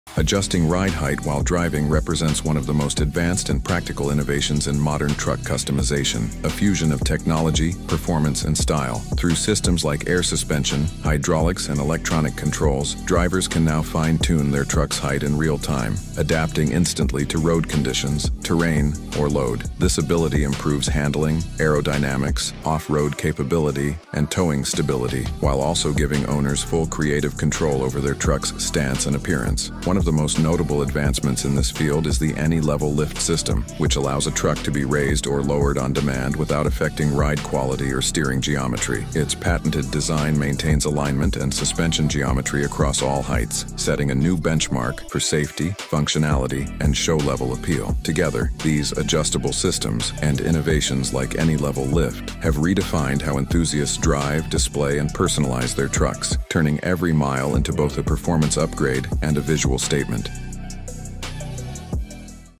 Adjustable Ride Height - The AnyLevel Lift
- Using air, hydraulic, or electronic suspension technologies, drivers can now raise or lower their trucks in real time to adapt to changing road conditions, terrain, or personal preference.
- This capability enhances aerodynamics, off-road clearance, towing stability, and even accessibility while allowing enthusiasts to perfect their stance for shows or cruising.